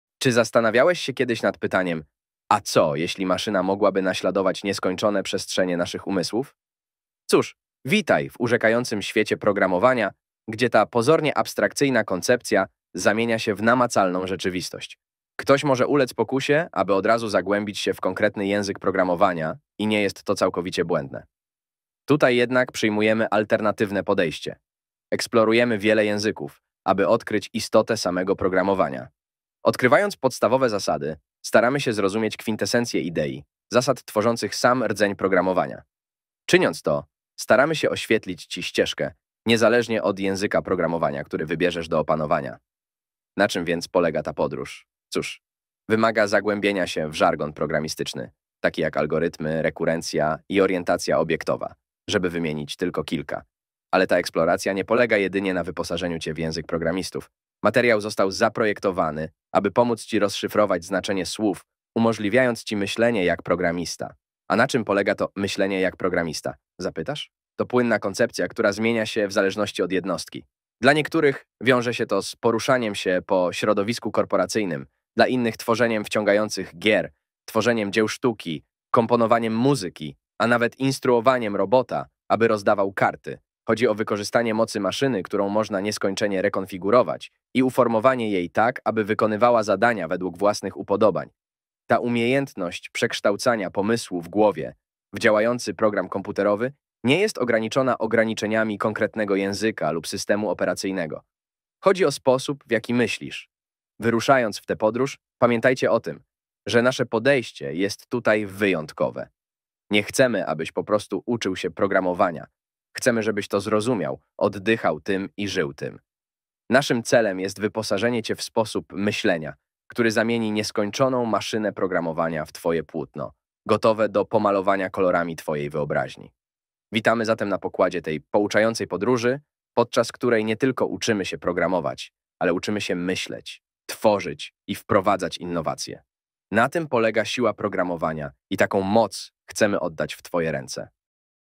Odblokowanie mocy programowania: Naucz się myśleć jak programista
Witamy w urzekającym świecie programowania, w którym zgłębiamy koncepcję maszyn naśladujących nieskończone przestrzenie naszych umysłów. Dołącz do nas w wyjątkowej podróży, podczas której odkrywamy istotę programowania w różnych językach, a naszym celem jest wyposażenie Cię w sposób myślenia, który przekształci nieskończoną maszynę programowania w Twoje płótno wyobraźni. Naucz się myśleć, tworzyć i wprowadzać innowacje, zagłębiając się w podstawowe zasady programowania. Zapisz się teraz, aby wyruszyć w tę pouczającą podróż!

more...

CZYM JEST PROGRAMOWANIE
Wprowadzenie Dla Osób Nietechnicznych